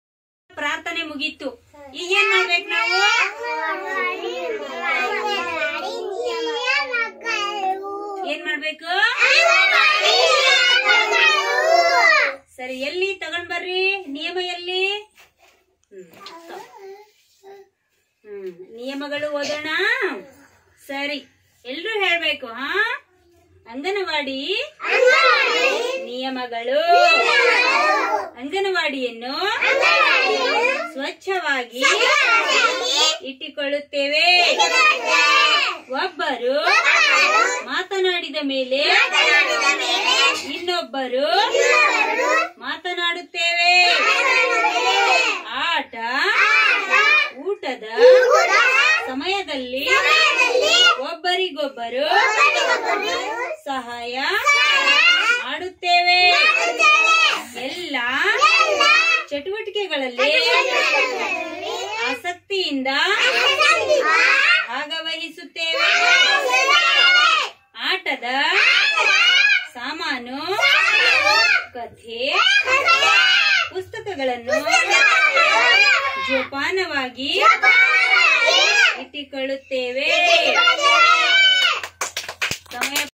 ಅಂಗನವಾಡಿ, ನಿಯಮಗಳು ಮಕ್ಕಳಿಗೆ ಹೇಗೆ ಹೇಳಿ ಕೊಡಬೇಕು, ಎಂದು ಈ ವಿಡಿಯೋದಲ್ಲಿ, ಸಂಪೂರ್ಣವಾಗಿ ನೋಡಿ (Anganawadi Rules
ಅಂಗನವಾಡಿ ನಿಯಮಗಳು, ಅಂಗನವಾಡಿ, ಕೇಂದ್ರದಲ್ಲಿ, ಮಕ್ಕಳಿಗೆ ಹೇಳಿ ಕೊಡುವುದು ಹೇಗೆ

ಅಂಗನವಾಡಿಯನ್ನು, ಸ್ವಚ್ಛವಾಗಿ ಇಟ್ಟುಕೊಳ್ಳುತ್ತೇವೆ,ಒಬ್ಬರು ಮಾತನಾಡಿದ ಮೇಲೆ ಇನ್ನೊಬ್ಬರು ಮಾತನಾಡುತ್ತೀವೆ, ಆಟ - ಊಟದ ಸಮಯದಲ್ಲಿ ಒಬ್ಬರಿಗೊಬ್ಬರು ಸಹಾಯ ಮಾಡುತ್ತೇವೆ,
ಎಲ್ಲಾ ಚಟುವಟಿಕೆಗಳಲ್ಲಿ, ಆಸಕ್ತಿಯಿಂದ ಭಾಗವಹಿಸುತ್ತೇವೆ,
ಎಲ್ಲಾ ಆಟದ ಸಾಮಾನು ಕಥೆ, ಪುಸ್ತಕಗಳನ್ನು,ಜೋಪಾನವಾಗಿ, ಇಟ್ಟುಕೊಳ್ಳುತ್ತೇವೆ,